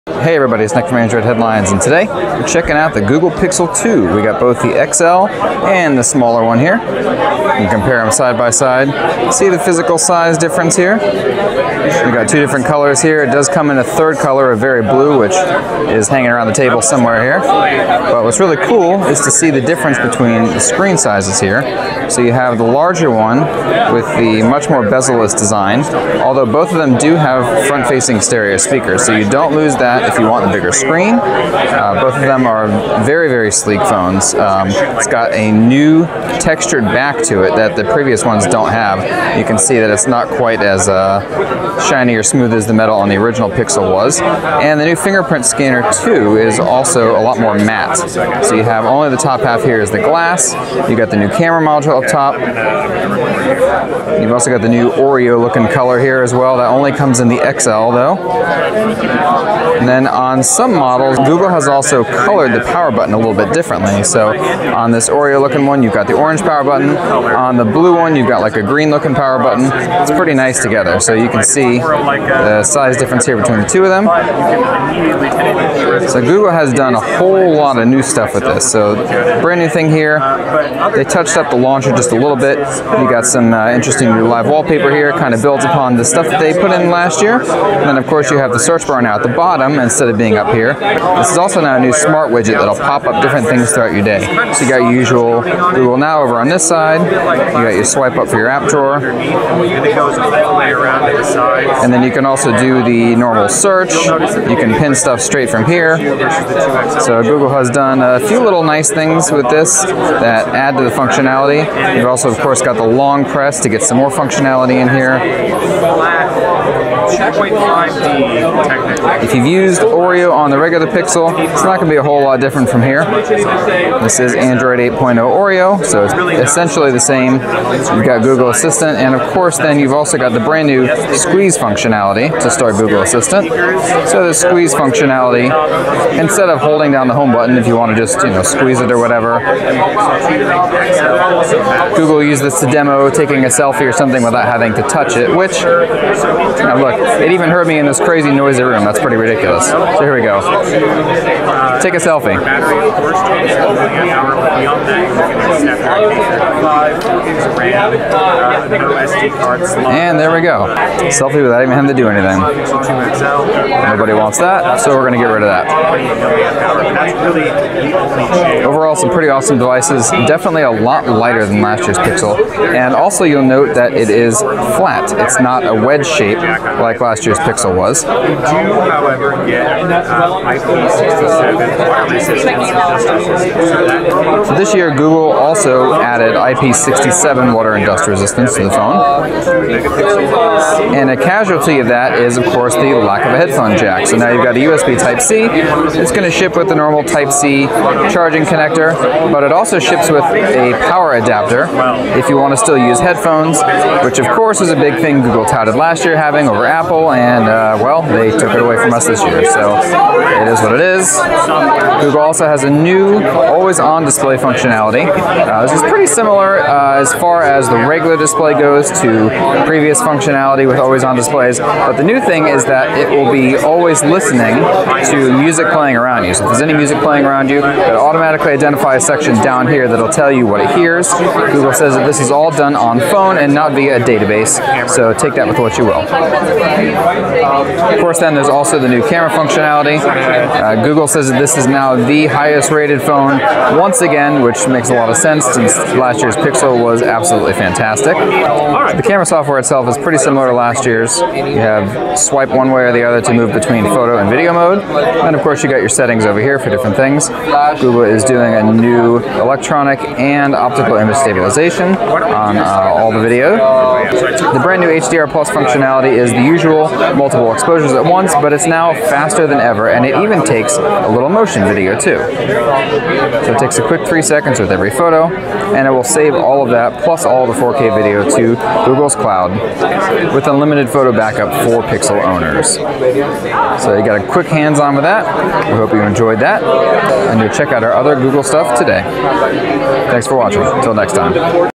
Google Pixel 2 Hands On
The Google Pixel 2 and Pixel 2 XL are here at last, looking just like the leaks but still sporting plenty of surprises.  Both models feature front-facing stereo speakers, a huge upgrade from last year's models, as well as beautiful new OLED screens. The Pixel 2 XL even features a new 6-inch P-OLED panel with curved edges on the glass, and of course both sport the latest in mobile processing specs and power. Google is touting a brand new camera experience that's not just built from the ground up for low light capabilities, but also for AR capabilities too. Google's ARCore is at the root of these new features, and this combined with new camera hardware is bringing a brand new AR experience to the table. There's even the addition of hardware stabilization on the camera this time around, which still works hand in hand with the excellent software stabilization Google brought last year. Be sure to check out everything else we've covered at the 2017 Made By Google event too, as Google has added boatloads of new products to the market.